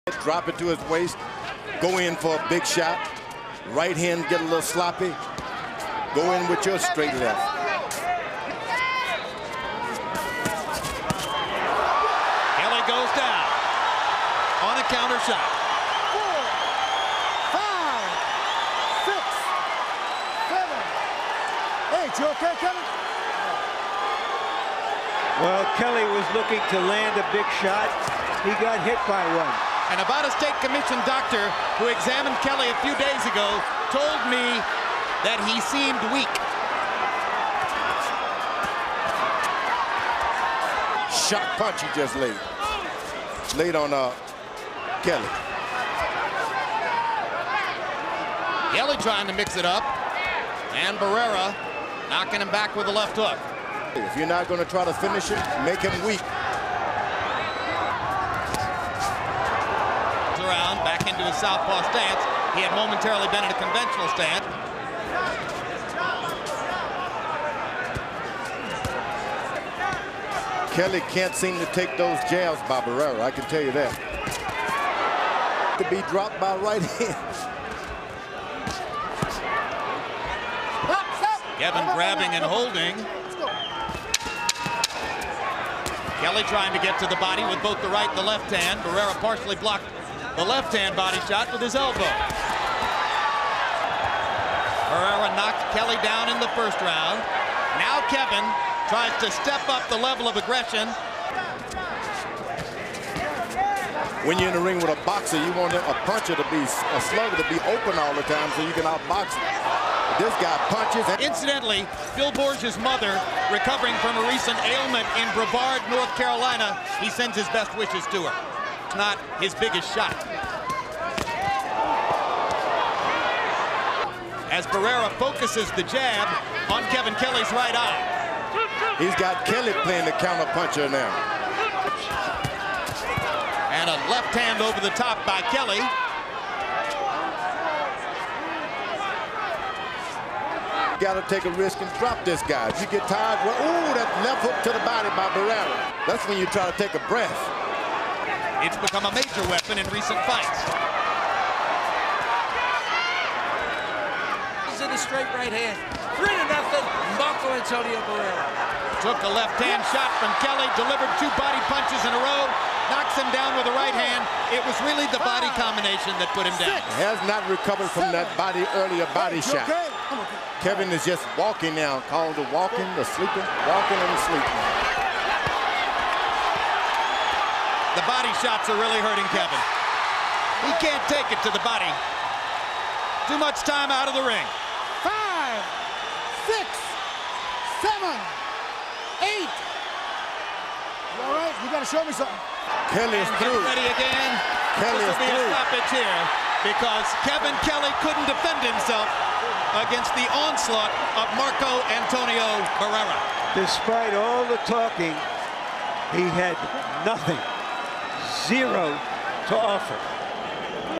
Marco Antonio Barrera vs Kevin Kelley (Highlights) 4K
Marco Antonio Barrera (56-3-0) vs Kevin Kelley (54-5-2)
12 Rounds
Feather
MGM Grand, Las Vegas, Nevada, USA | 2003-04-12